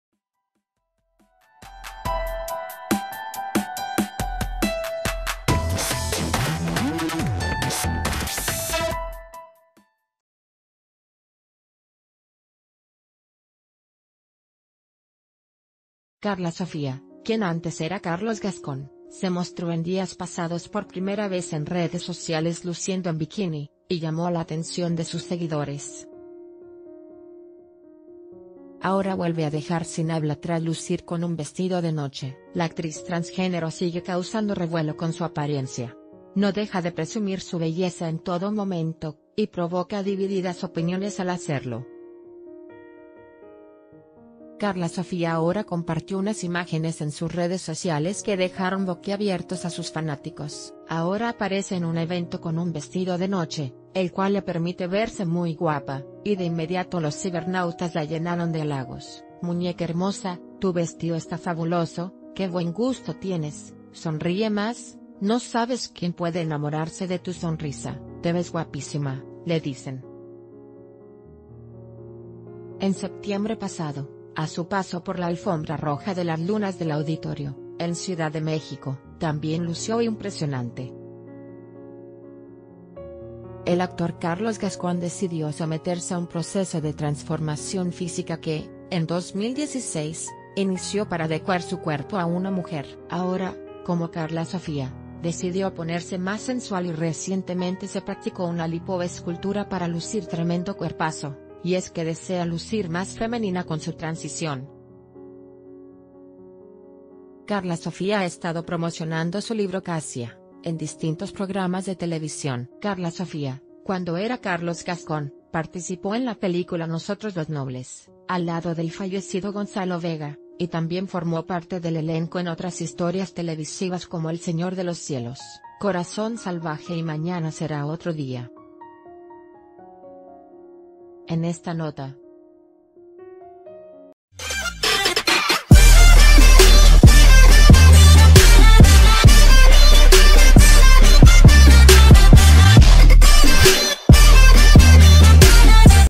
El look que Karla Sofía enseña ahora, paraliza la red
Ahora vuelve a dejar sin habla tras lucir con un vestido de noche. La actriz transgénero sigue causando revuelo con su apariencia. No dej...